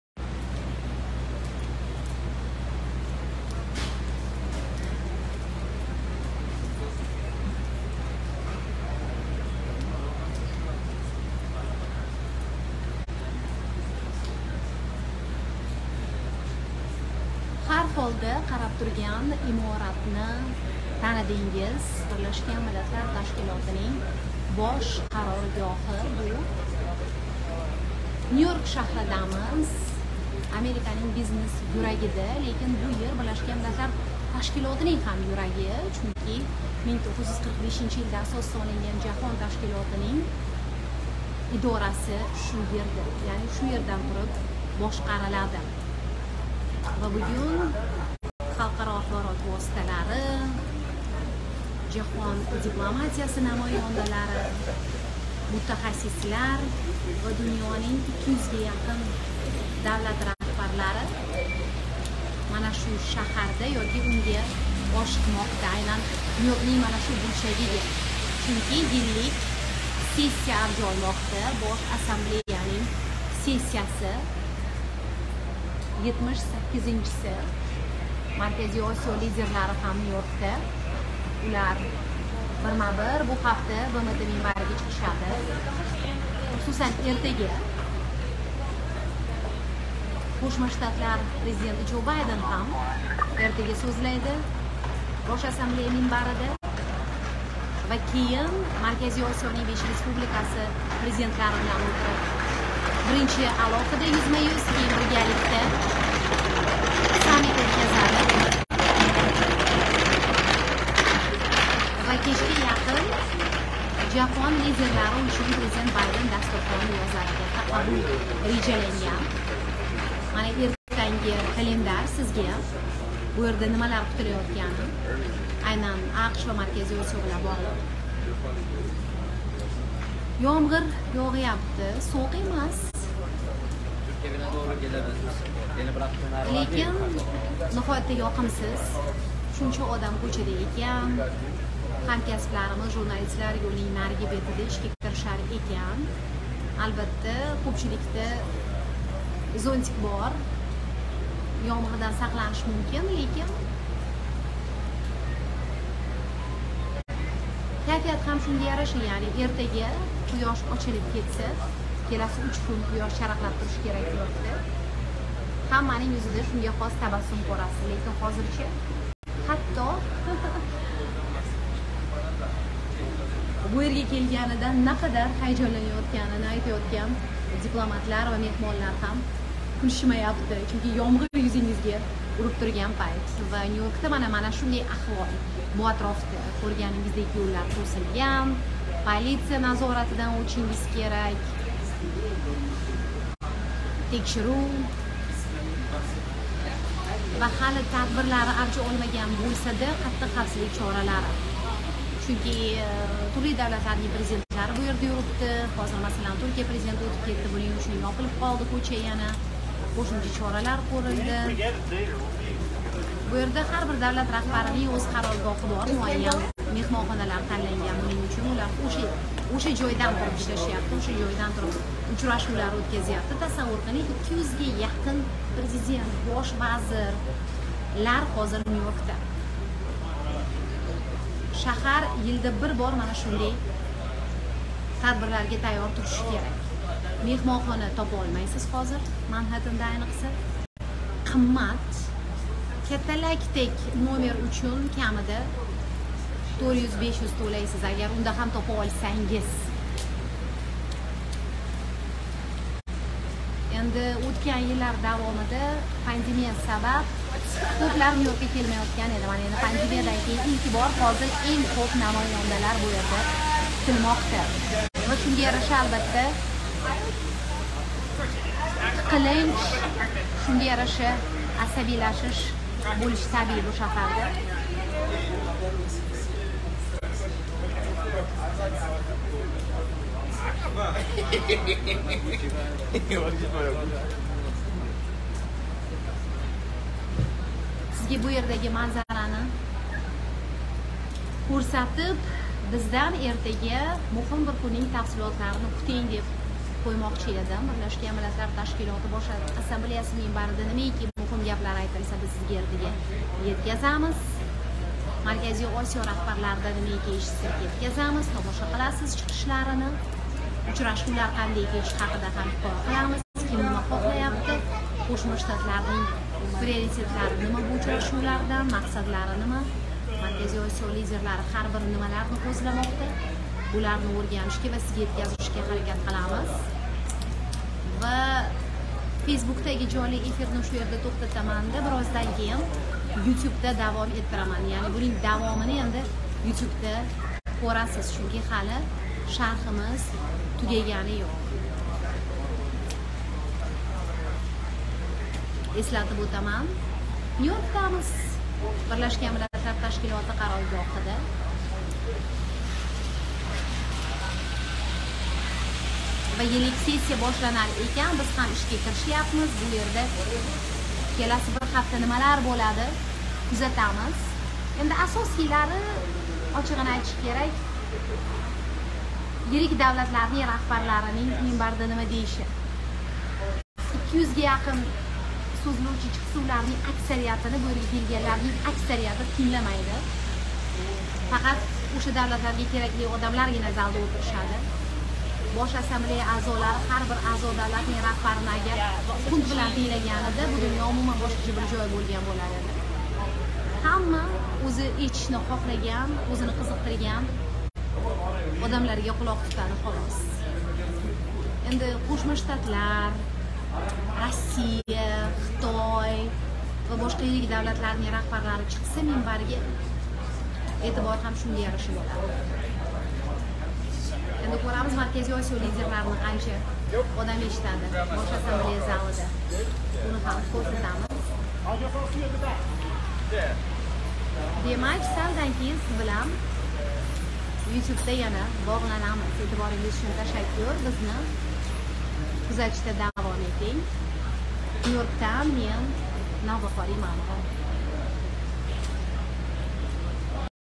Nyu-York: Dunyo BMT qarorgohida
Nyu-Yorkda BMT qarorgohida 200 ga yaqin davlat a'zo Bosh Assambleyaning yillik sessiyasi boshlanmoqda. 18-sentabrdagi manzara.